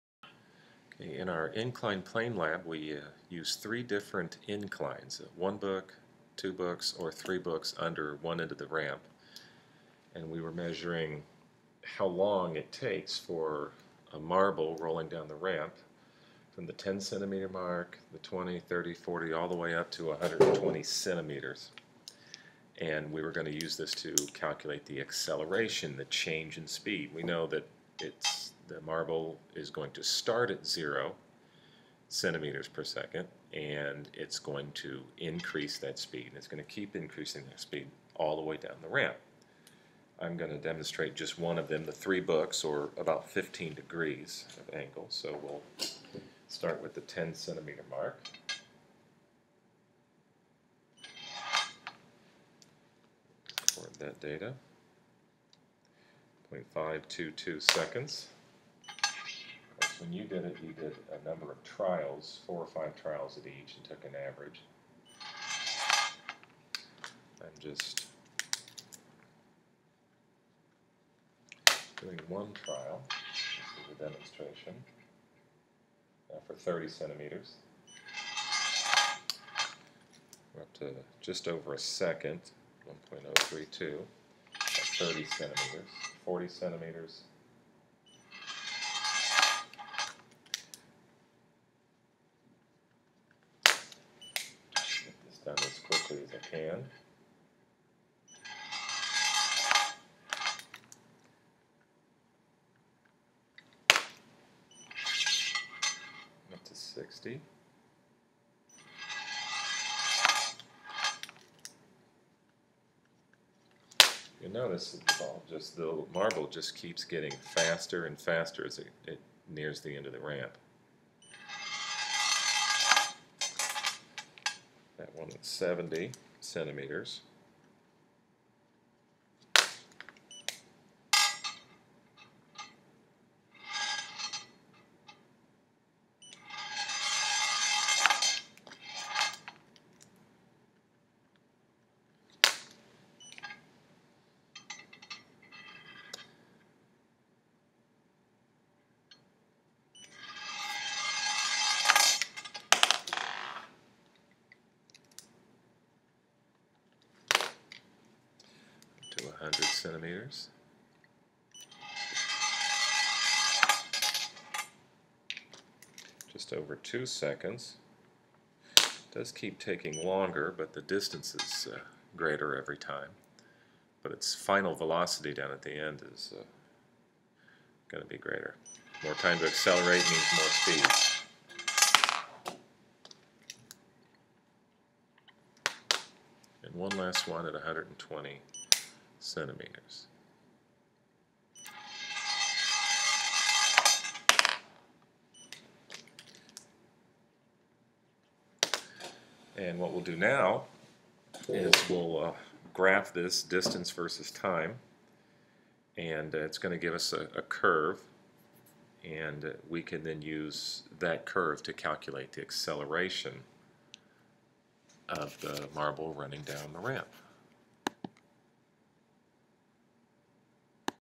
Inclined Plane Lab explanation
Mr. Snyder explains the inclined plane lab.